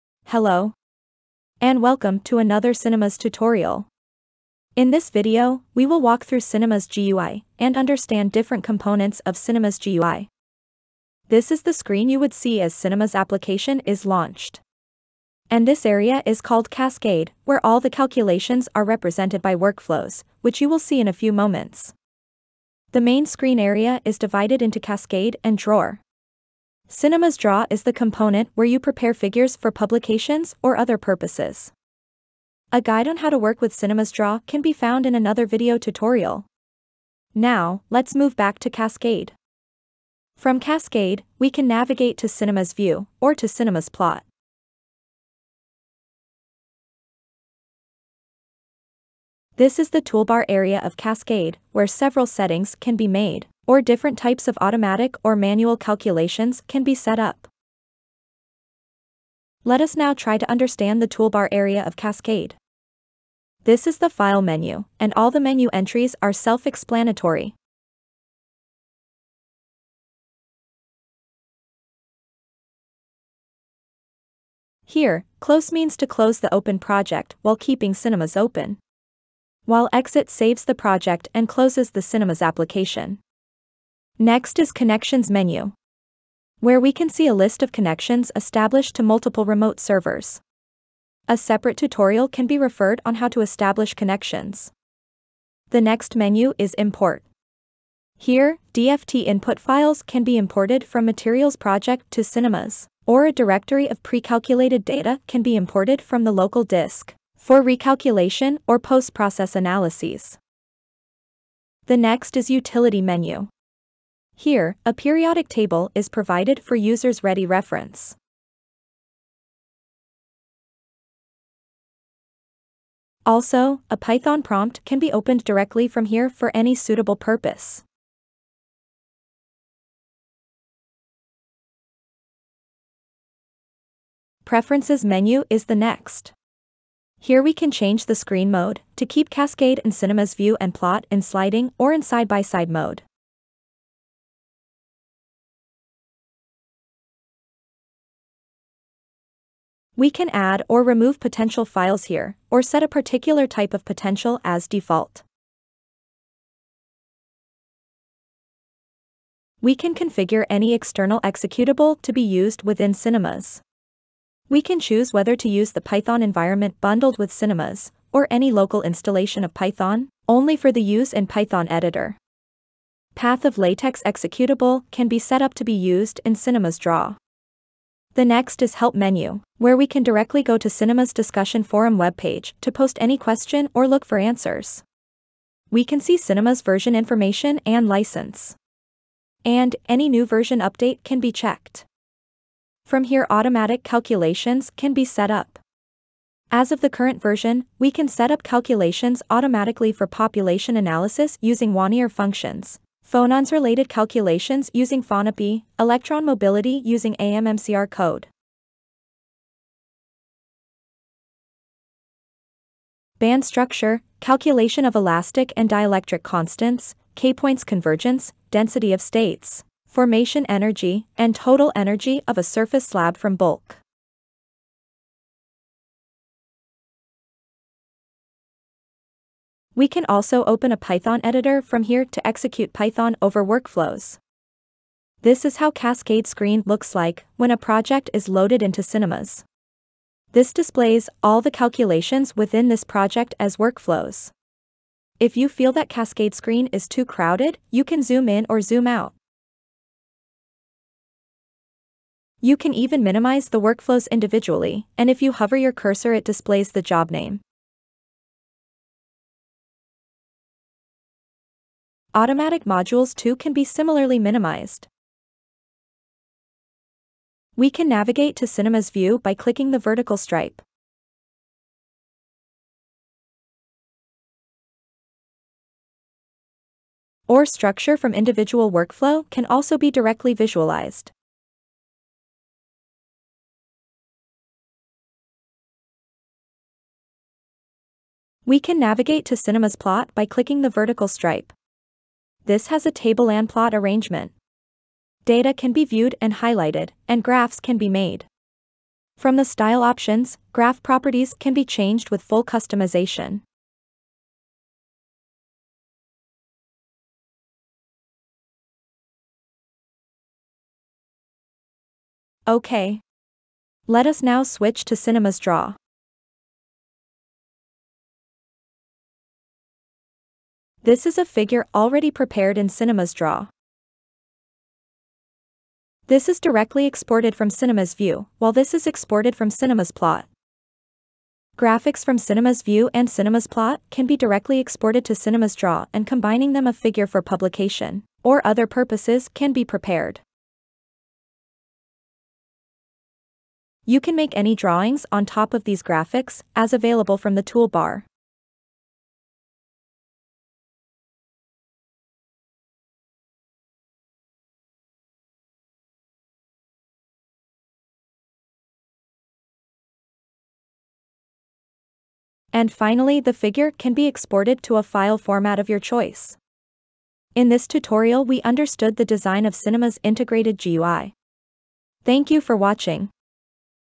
Understanding CINEMAS integrated GUI
This video explains how different components of CINEMAS are arranged through its integrated GUI.